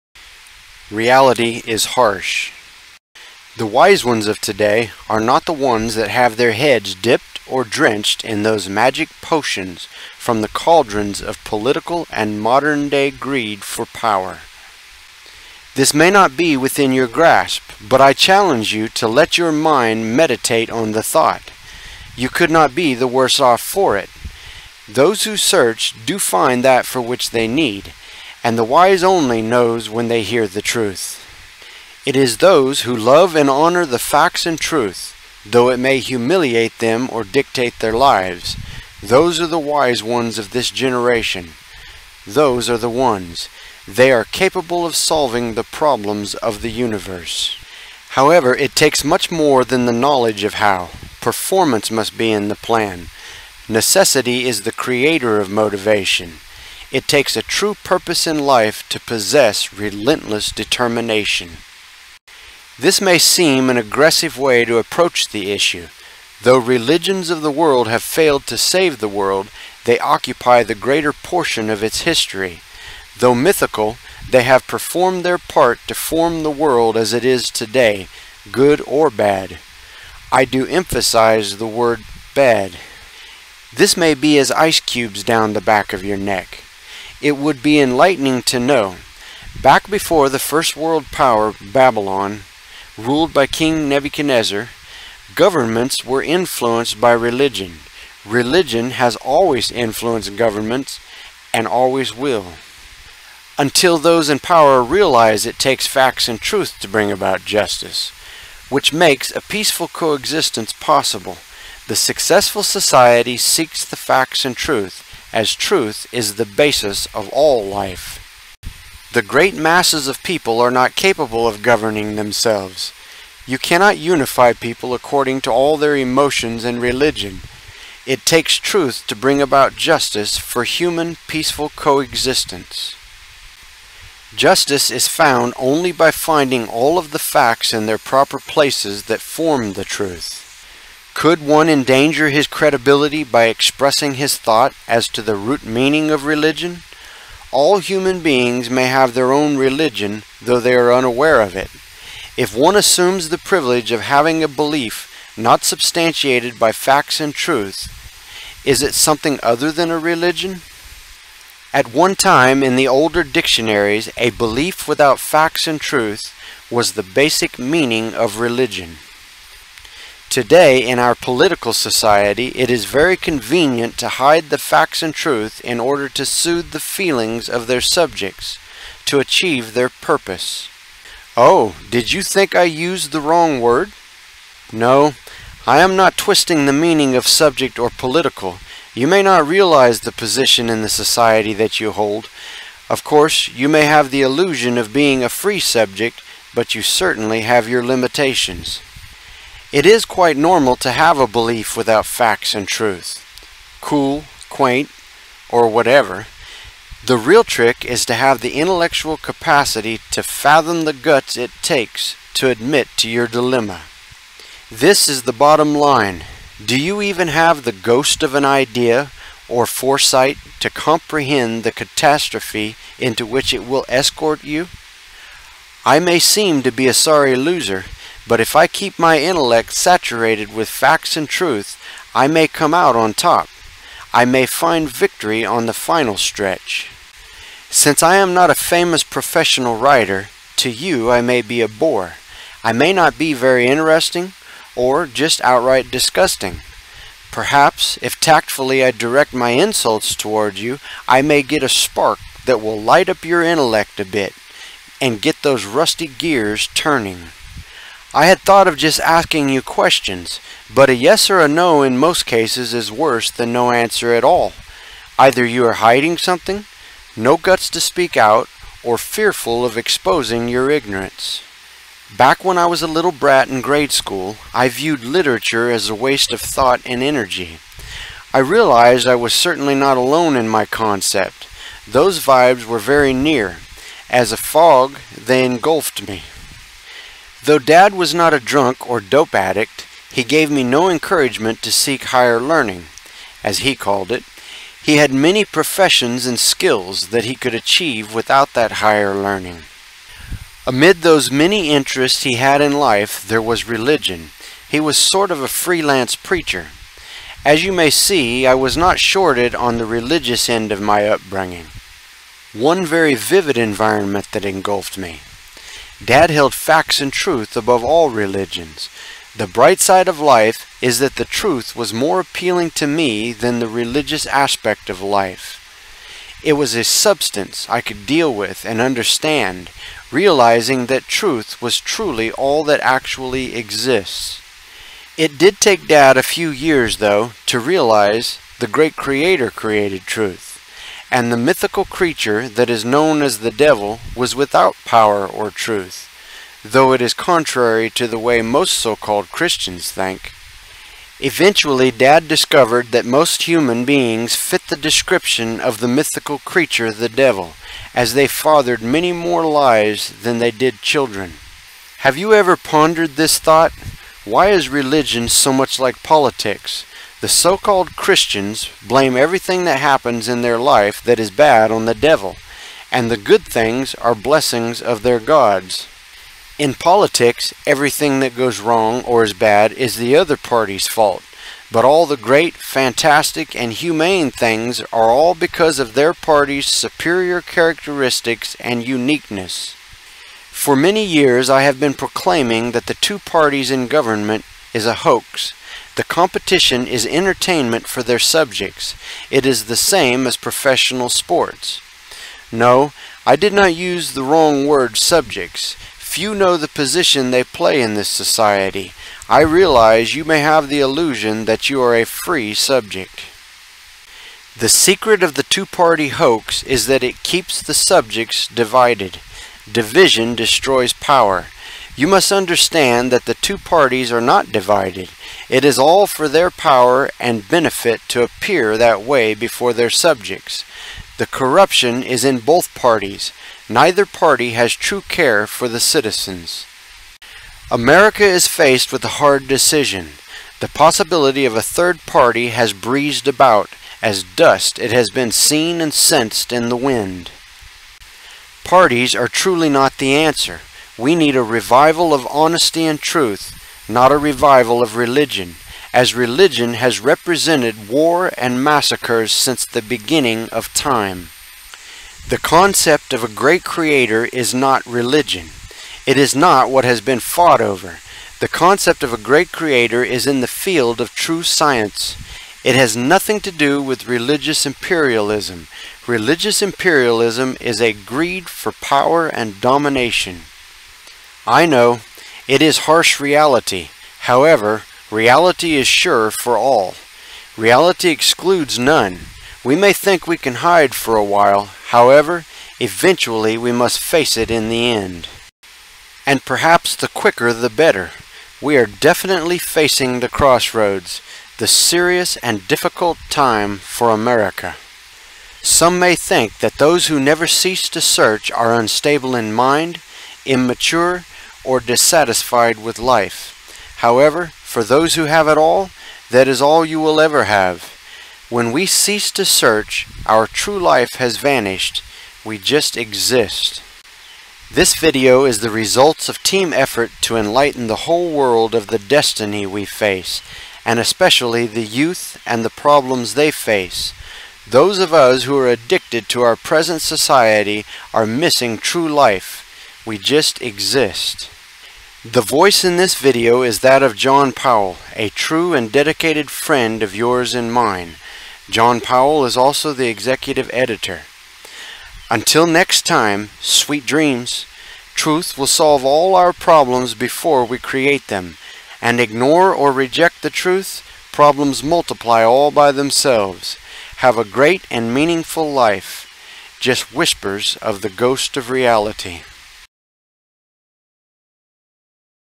America: will it recover its dignity
America: will it recover its dignity America is facing harsh reality. America is ignorant of the destiny it faces. That American Dream is only history. the American dream is no more a reality. Can America's great image be restored?  that  is the Serious Question.